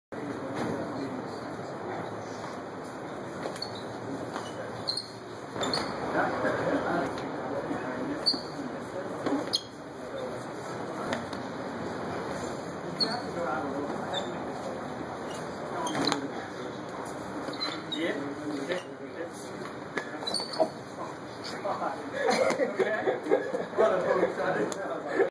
Omar Mora Wing Chun Practice
Omar Mora practicing Wing Chun drill